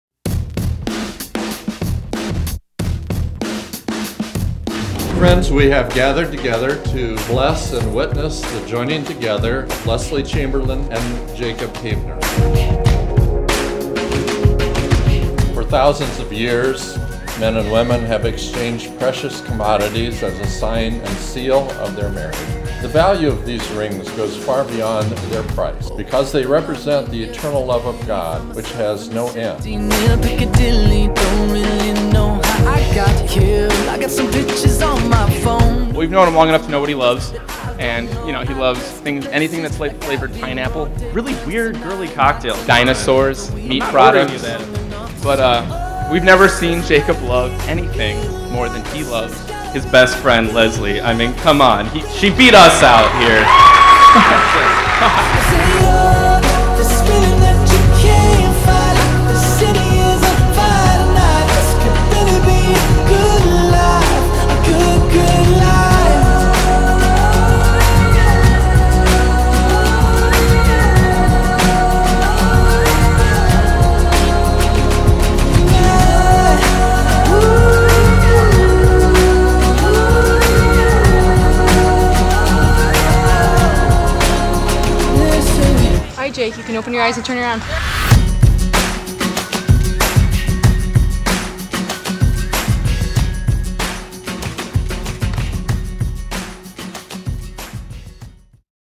Jake & Leslie's Wedding Trailer
Jake and Leslie were married at The Allure (AllureOne.com) in LaPorte, IN on November 19th.

These guys knew how to have a great time. You spend just a few minutes around the two of them and you can tell they are truly best friends and meant to be. Congratulations to the new Mr. & Mrs. Havner!!

We were able to provide DJ, Custom Venue Lighting and Cinematography for this rockstar couple.

Feature film to follow!

Tell us what you think and like us at:
Facebook.com/27Entertainment